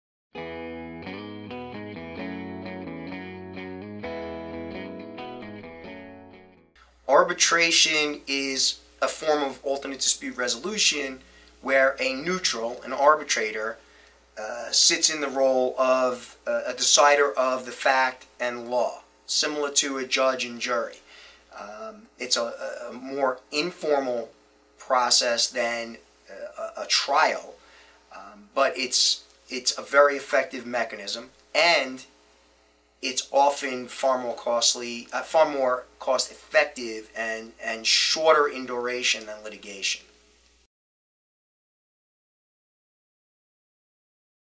What is arbitration?
Arbitration is a form of alternative dispute resolution where a neutral, an arbitrator, sits in the role of a decider of the fact and law, like a judge and jury. It's a more informal process then a trial. But it's a very effective mechanism and it's often far more cost effective and shorter in duration then litigation.